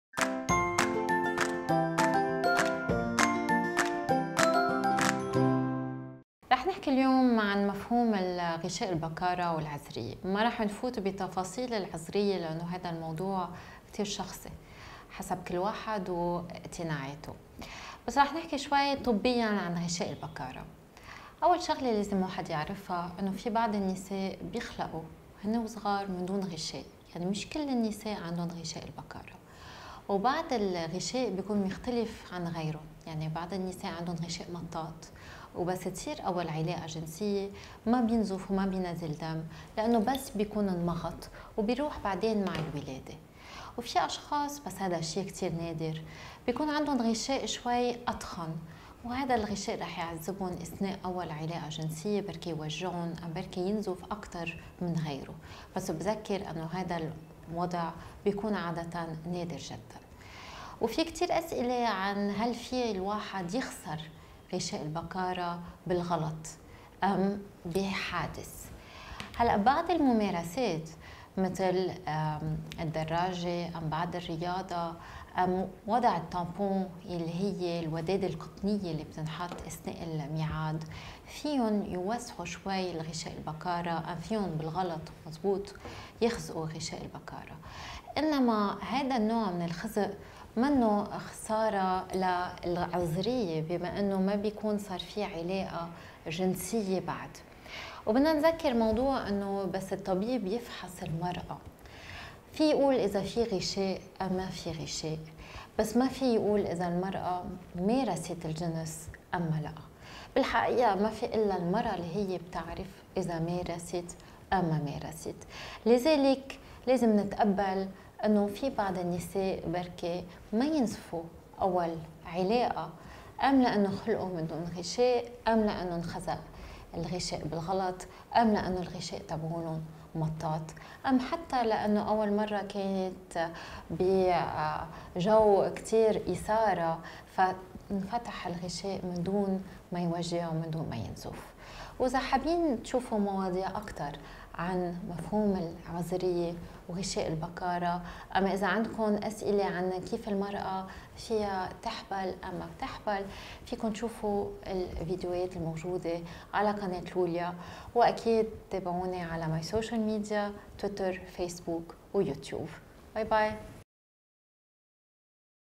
هل يمكن معرفة ما اذا كانت الفتاة عذراء أو لا؟
فيديو قديم عن العذرية. دعونا نتذكر أن العذرية  بناء اجتماعي. الغشاء أو إكليل المهبل ليس دليلاً على العذرية ولا يستطيع أحد حتى الطبيب أن يعرف إذا كانت المرأة مارست الجنس أم لا.